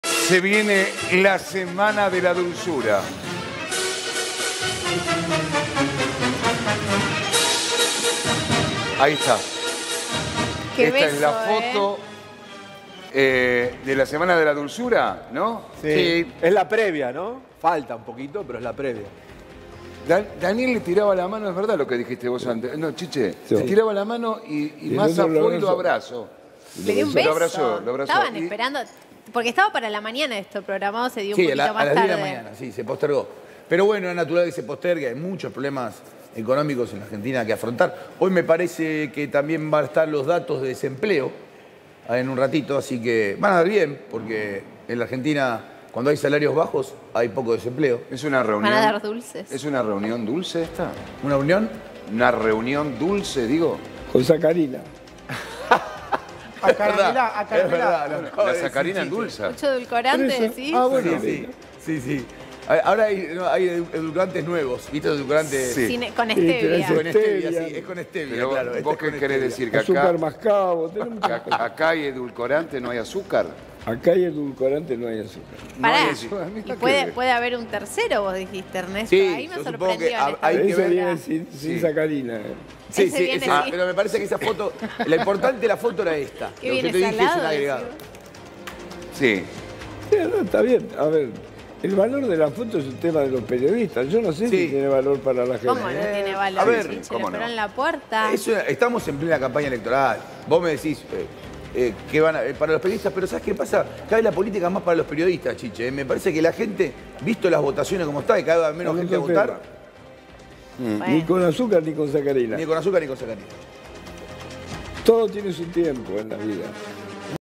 Así fue el reencuentro de Sergio Massa y Daniel Scioli después del cierre de listas para las PASO
El precandidato presidencial Sergio Massa recibió este jueves al mediodía al embajador en Brasil, Daniel Scioli. El encuentro tuvo lugar en el Palacio de Hacienda.

La entrevista fue tomada como una muestra de unidad del oficialismo, luego de los chispazos que se produjeron la semana pasada, cuando se cerraron las listas de precandidatos para las primarias abiertas, simultáneas y obligatorias (PASO) a desarrollarse el 13 de agosto próximo.